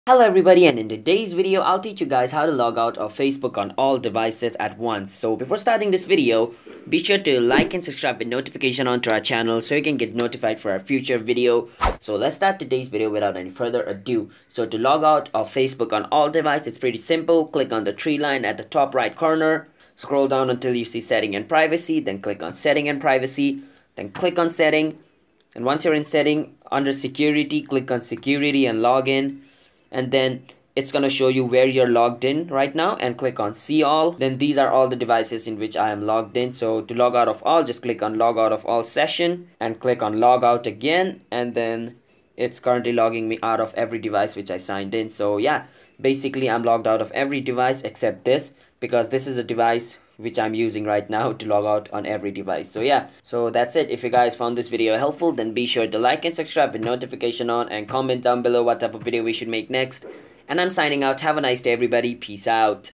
How to Logout of Facebook On All Devices | Facebook Log Out
Logout of Facebook on all devices 2021

How to logout of Facebook on all devices at once in 2021? This video walks you through the exact process on how to sign out of fb account on all devices. If you forgot to logout of your Facebook account on other's device, this is the best way to be secure. Once you do this, your account will be automatically logged out from all the devices. Also, if you doubt the unauthorized login from new devices, logout of all devices is the only thing you need to do. So, watch the video till the end to learn the trick in detail.

Most of people might get a login to their multiple devices so to stop others to use your Facebook must be log out from that device. Watch our full video to learn the easy process on how to.
1: Open Facebook on your device.
2: Go to the Settings.
3: Tap on Security and Login.
4: Click on See All.
5: Scroll down and click on “Log out from all sessions”.
6: Tap on Log out.
Then those multiple devices where you have been log in Facebook gets log out.